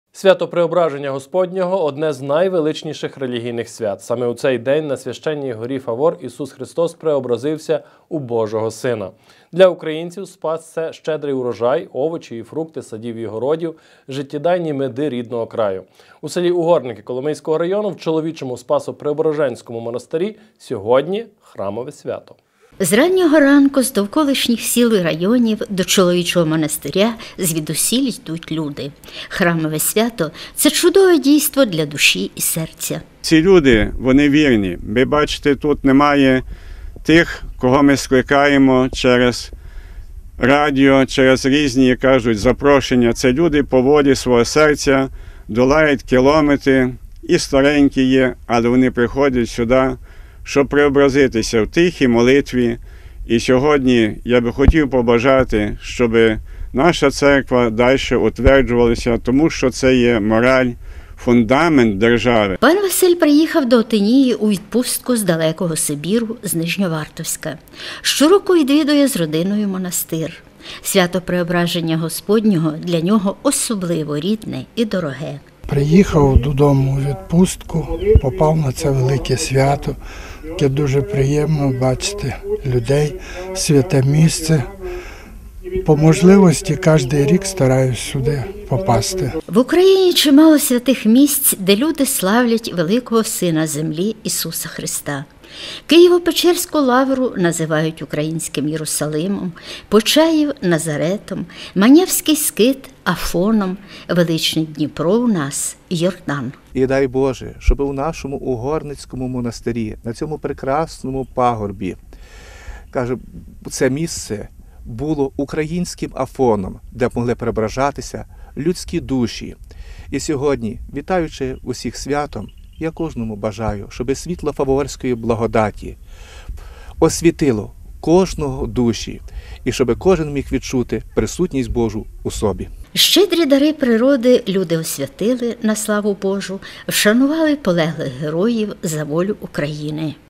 Свято Спаса в Угорницькому монастирі
Свято Преображення Господнього — одне з найвеличніших свят українського народу. Саме в цей день на Священній горі Фавор, Ісус Христос преобразився в Божого сина. Для українців — Спас — це щедрий урожай, овочі і фрукти садів і городів, життєдайні меди рідного краю. В селі Угорники Коломийського району в чоловічому Спасо-Преображенському монастирі — сьогодні храмове свято.